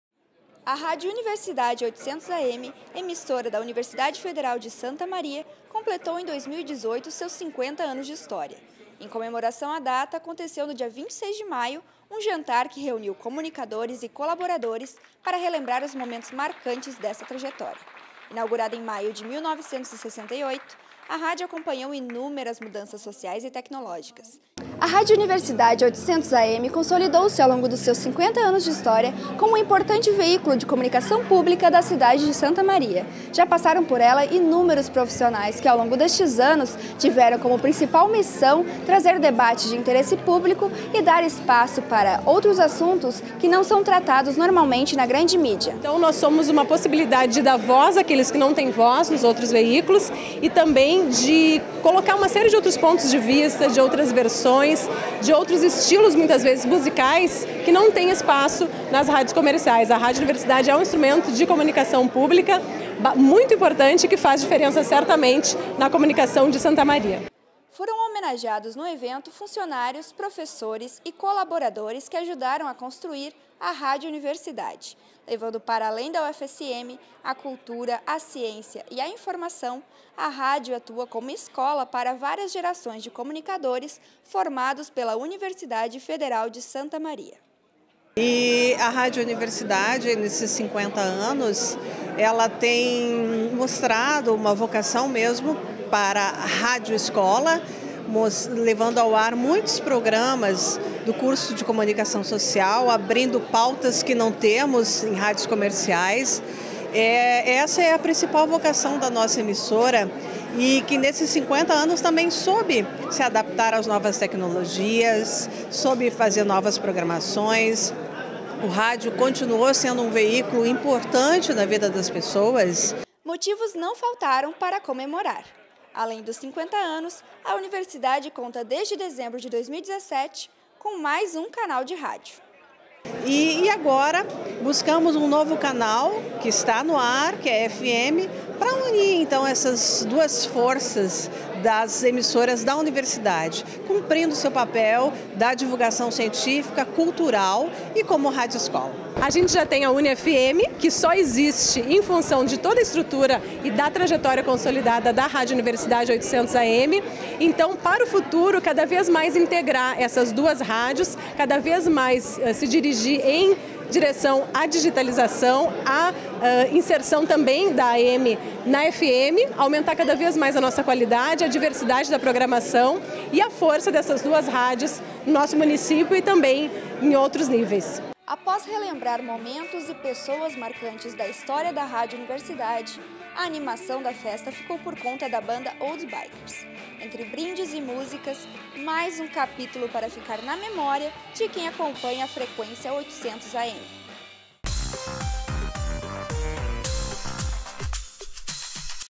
Jantar de 50 Anos da Rádio Universidade - Acontece na UFSM
Lembranças, homenagens e celebração. Teve isso e muito mais no jantar comemorativo aos 50 anos da Rádio Universidade! Entre brindes e música, mais um capítulo para ficar na memória de quem acompanha a frequência 800 AM em Santa Maria e região.